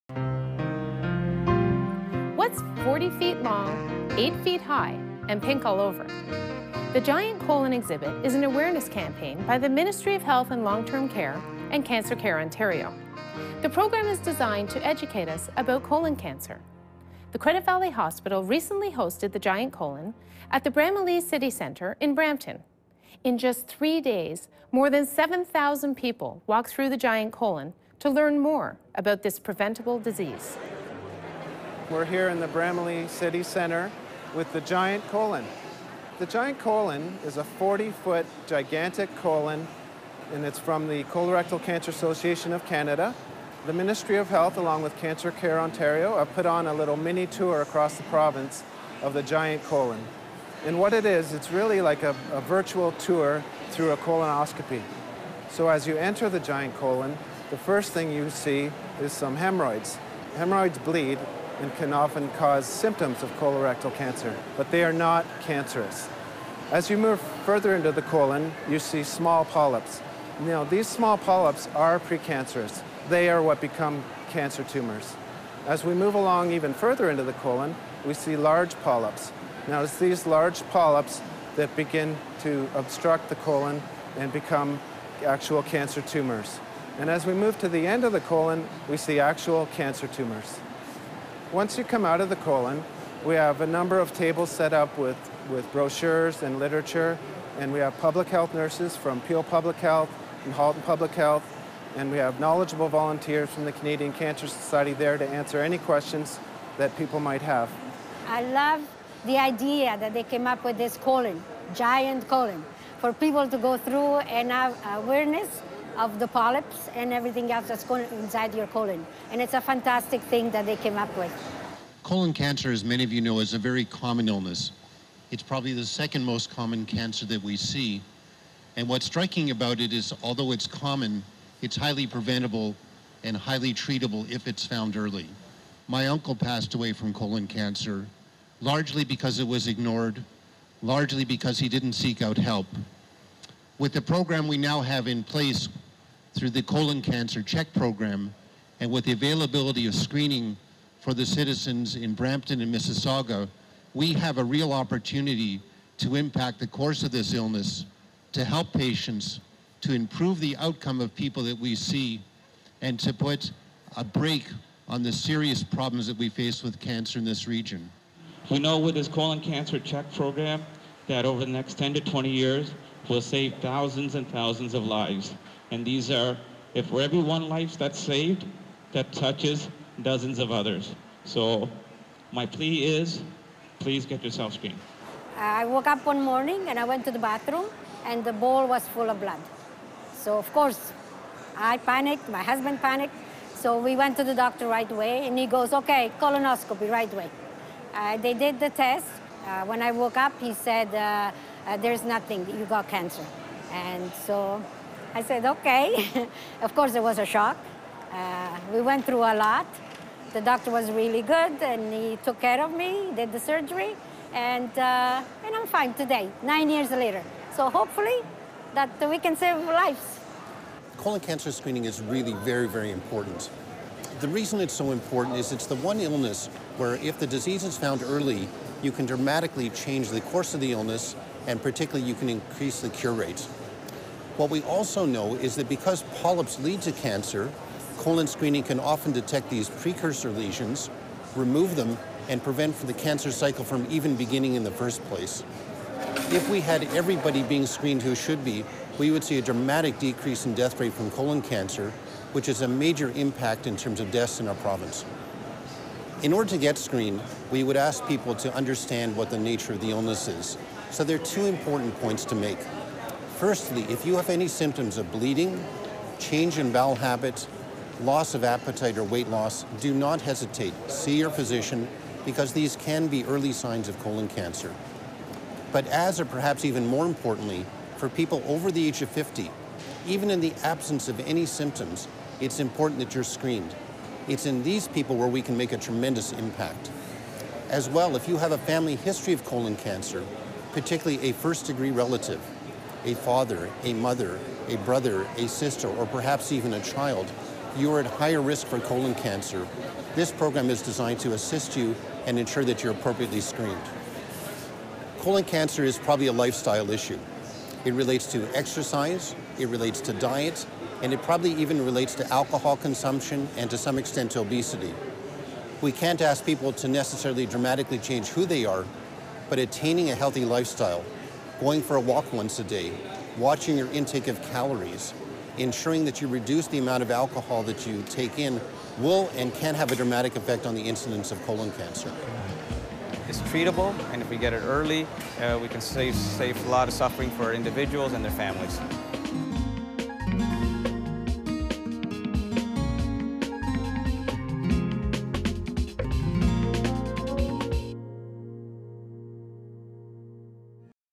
Credit Valley Contact - 201005 - part 5 -Giant Colon Exhibit.
Take a tour through the Giant Colon and find out what you need to know about colorectal cancer. In 2010, ColonCancerCheck sponsored an Ontario tour of the Giant Colon to raise awareness of the importance of early colorectal cancer screening.